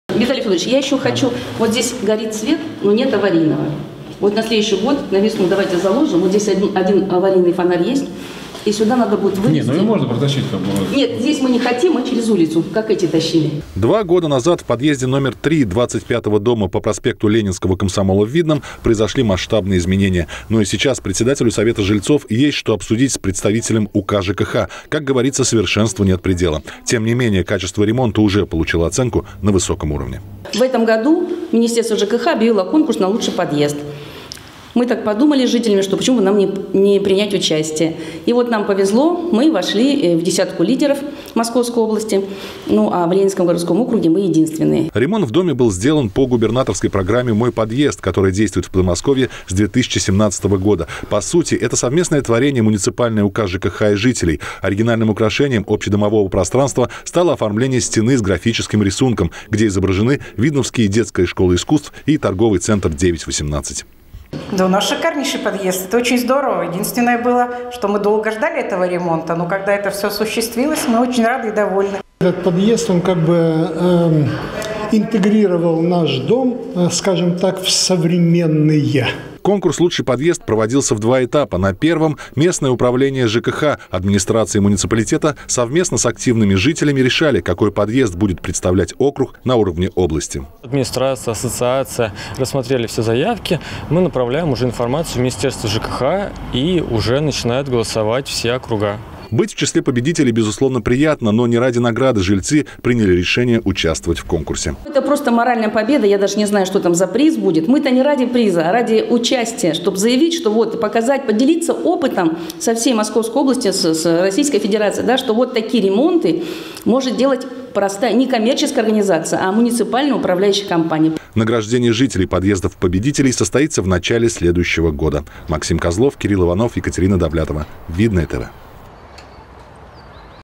ШИК, БЛЕСК,КРАСОТА!
В Министерстве жилищно-коммунального хозяйства Московской области назвали победителей конкурса «Лучший подъезд Подмосковья». В десятку лучших вошёл и подъезд №3 в доме №25 по проспекту Ленинского комсомола в Видном. Что же в нём покорило конкурсную комиссию, узнал Максим Козлов.